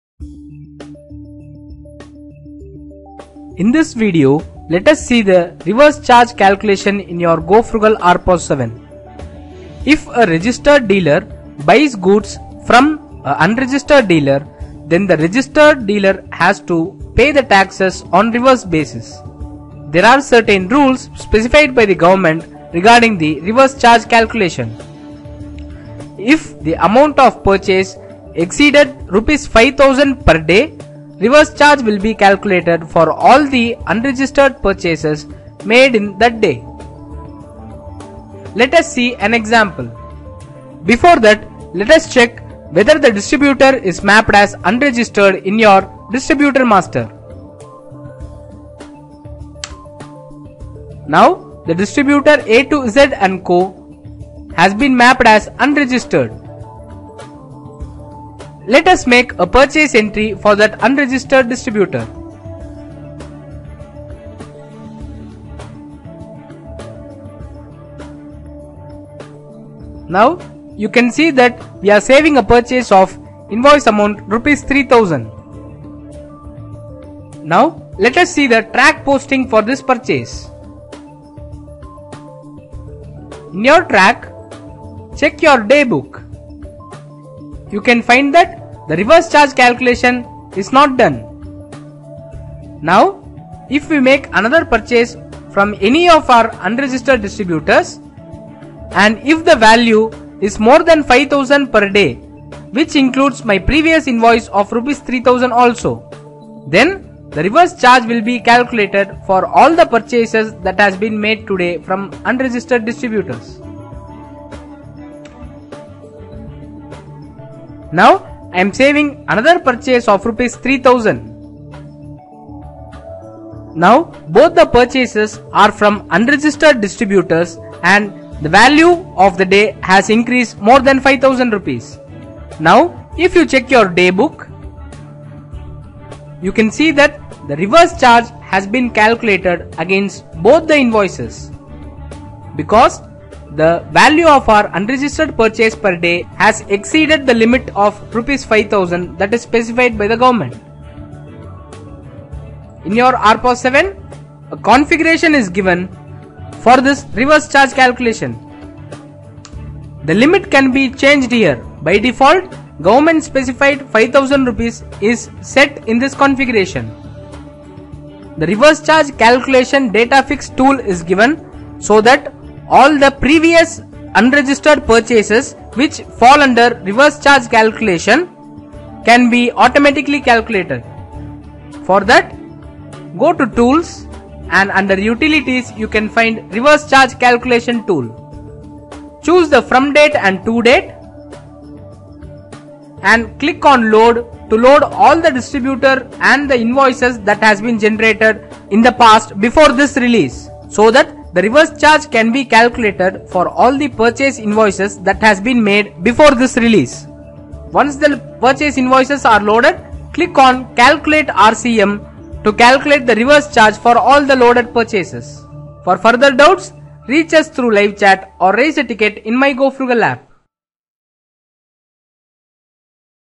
GoFrugal RPOS7 - Reverse Charge Calculation - Unregistered Purchase
Reverse tax calculation in Purchase : What is it?

As per the GST regime, if a registered person purchases goods from an unregistered dealer(URD) then the registered taxpayer is liable to pay GST on reverse charge basis. All the provisions of the Act will apply to such recipient as if he were the person liable for paying the tax in relation to the supply of goods.

How to?

If the distributor master GST Reg type is selected as un-registered then that distributor is URD. Any purchase made from these kind of distributors more than 5000 Rs/- per day then the buyer need to pay GST. Application will calculate this reverse charge by default from this release onwards, if that day URD purchase value is more than 5000 Rs/- & most importantly irrespective of the number of Purchases made for that day.

For the reverse charge applicable amount new configuration added in the product. Config : Tools -- Configuration -- Configuration -- Common settings -- GST config -- Enter reverse tax applicable limit - 5000(Default).

Data patch for existing entries : New menu will be provided in the application under the menu Tools -- Utilities -- Reverse charge calc tool. Users need to give the respective period and select the distributor or select all distributor and then click the "Load" button. What ever old purchase entries are applicable for reverse tax calculation will be loaded in screen automatically. After that click "Reverse RCM" button to calculate the reverse tax for those purchase's.